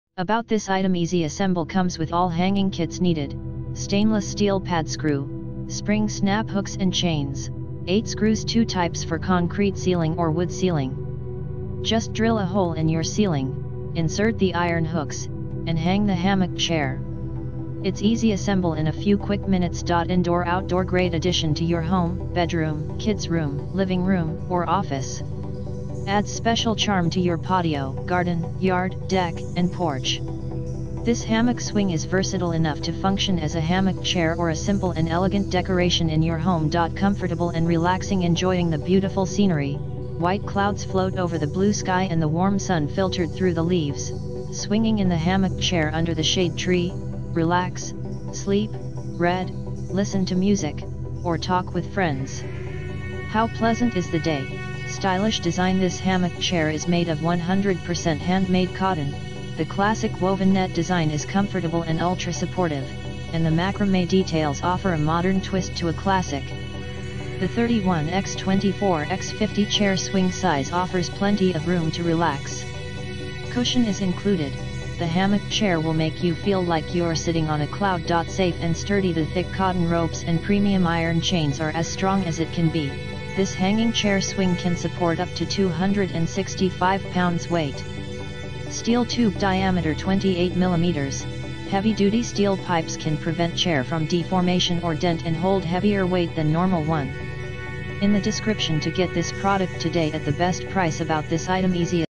Patio Watcher Hammock Chair Macrame Swing with Cushion and Hanging Hardware Kits, Handmade Knitted
Patio Watcher Hammock Chair Macrame Swing with Cushion and Hanging Hardware Kits, Handmade Knitted Mesh Rope Swing Chair for Indoor, Outdoor, Home, Bedroom, Patio, Yard，Deck, Garden

EASY ASSEMBLE-Comes with all hanging kits needed, stainless steel pad-screw, spring snap hooks and chains, 8 screws-2 types for concrete ceiling or wood ceiling. Just drill a hole in your ceiling, insert the iron hooks, and hang the hammock chair. It’s easy assemble in a few quick minutes.
INDOOR OUTDOOR-Great addition to your home, bedroom, kid’s room, living room or office. Adds special charm to your patio, garden, yard, deck and porch. This hammock swing is versatile enough to function as a hammock chair or a simple and elegant decoration in your home.
COMFORTABLE and RELAXING-Enjoying the beautiful scenery, white clouds float over the blue sky and the warm sun filtered through the leaves, swinging in the hammock chair under the shade tree, relax, sleep, read, listen to music, or talk with friends. How pleasant is the day!
STYLISH DESIGN-This hammock chair is made of 100% handmade cotton, the classic woven-net design is comfortable and ultra-supportive, and the macrame details offer a modern twist to a classic. The 31” x 24” x 50”chair swing size offers plenty of room to relax. Cushion is included, the hammock chair will make you feel like you’re sitting on a cloud.
SAFE and STURDY-The thick cotton ropes and premium iron chains are as strong as it can be, this hanging chair swing can support up to 265lbs weight. Steel tube diameter 28mm, heavy-duty steel pipes can prevent chair from deformation or dent and hold heavier weight than normal one.